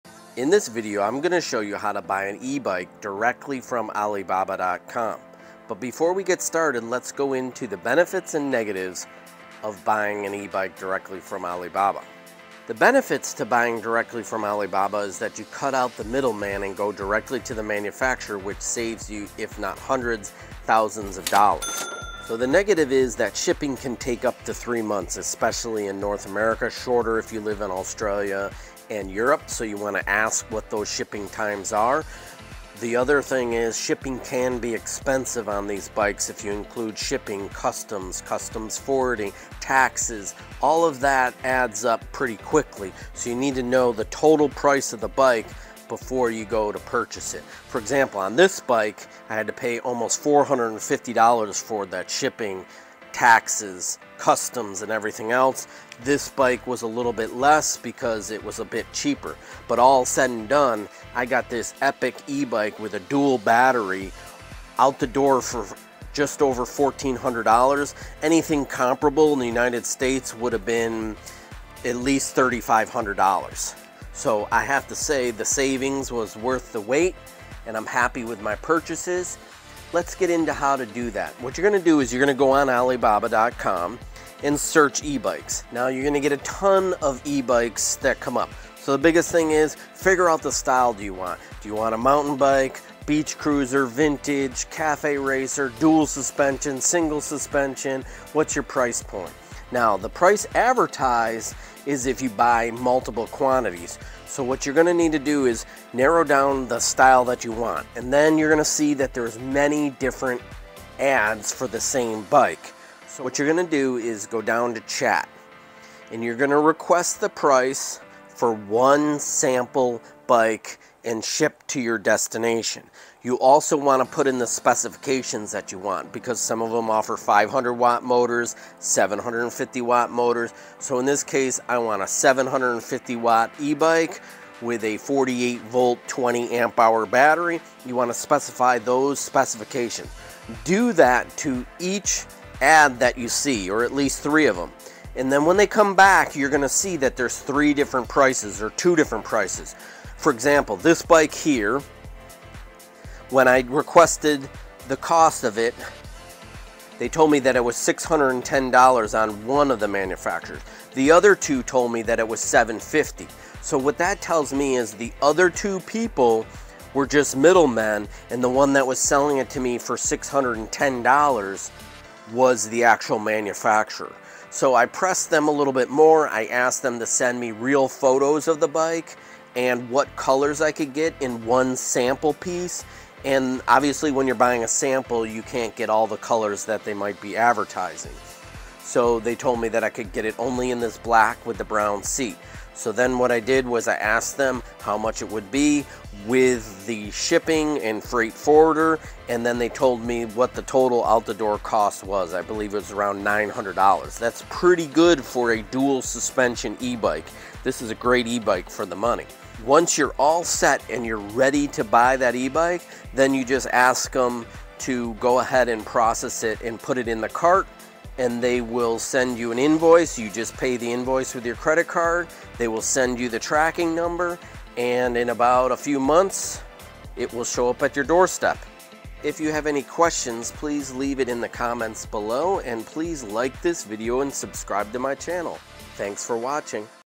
How to Buy eBike on Alibaba - save $100's or $1000's
This is quick video outlining some of the benefits and negatives on buying eBike on Alibaba.com and tutorial on how to purchase a bike from manufactures or sellers.

Chapters:
0:00 - Intro
0:12 - benefits (pros)
0:22 - negatives
1:20 - how to order
3:39 - conclusion